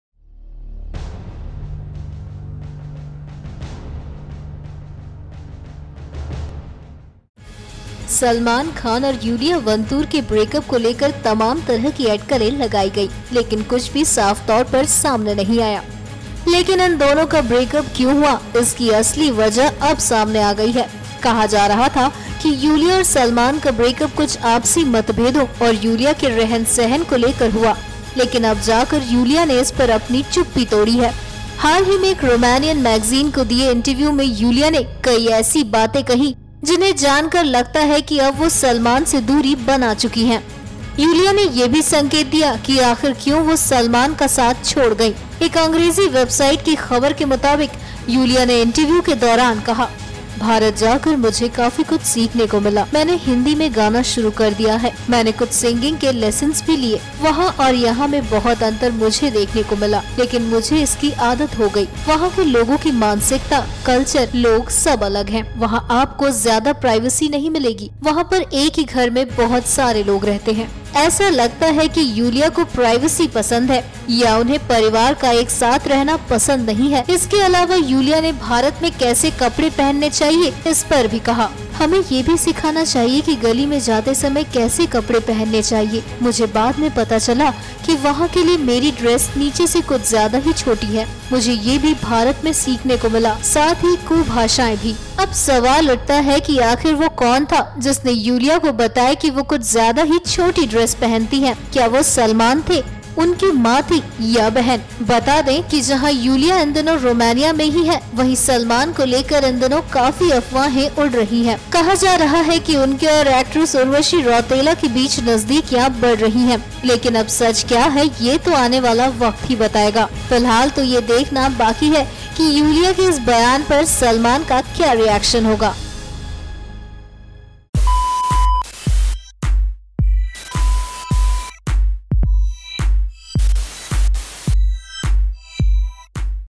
Iulia ने किया खुलासा ,की क्यों हुई Salman से अलग
Alot has been said and written about Salman Khan and his rumoured Romanian girlfriend Iulia Vantur –  from the rumours of them getting married this year, to they having a break up. While both of them remained tight lipped about their relationship, their public appearances gave many a chance to speculate that they are going strong. We had reported earlier that Iulia has gone back to Romania and that perhaps the couple has parted ways. In a recent interview Iulia opened up to a Romanian magazine about her experience in India.